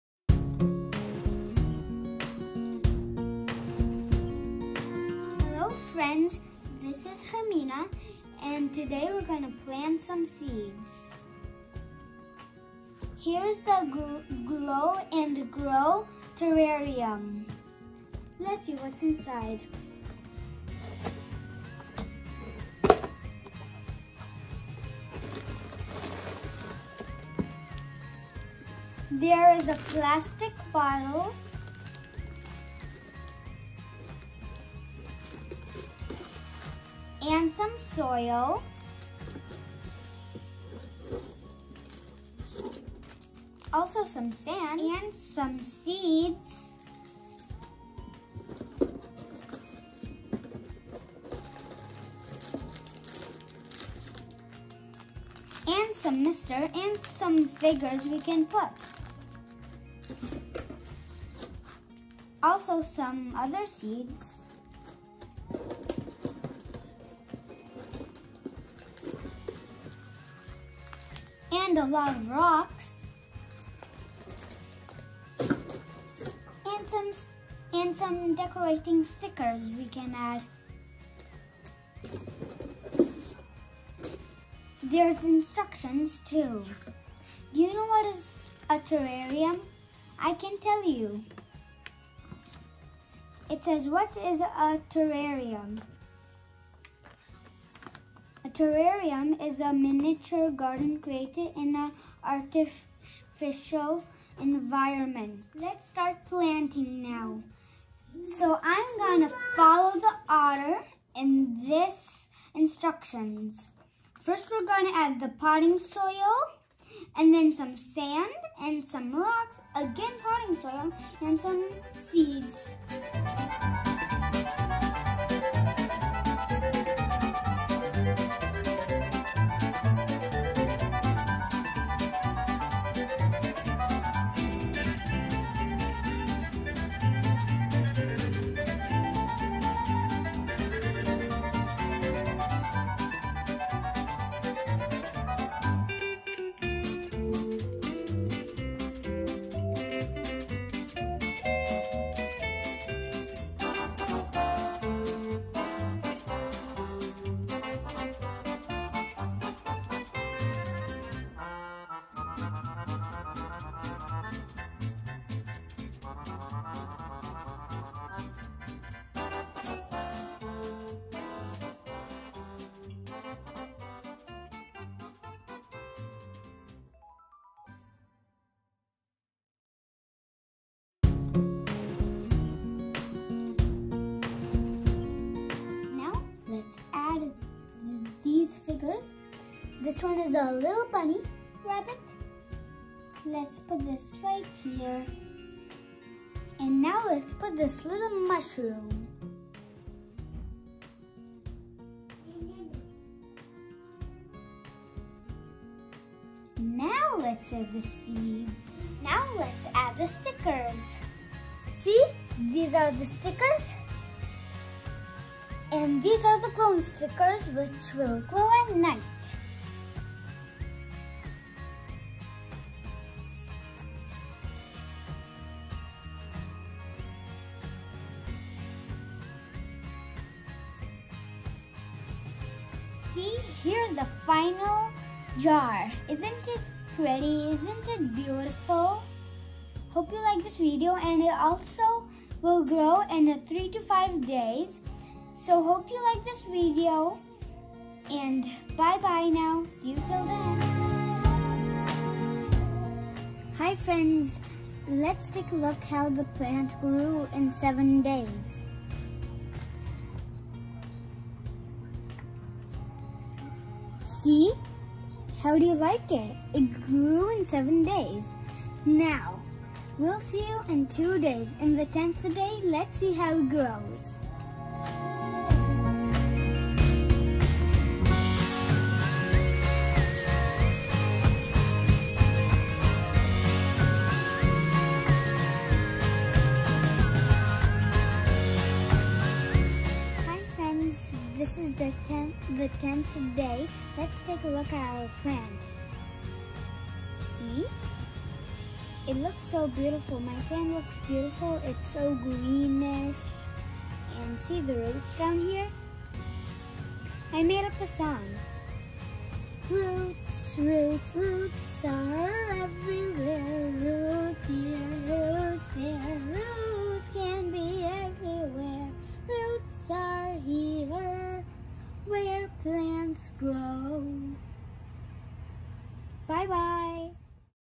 Glow and Grow Terrarium by me
Here I’m uploading a video for how to grow your own terrarium at home without anyone’s help.Hope you enjoy watching it💕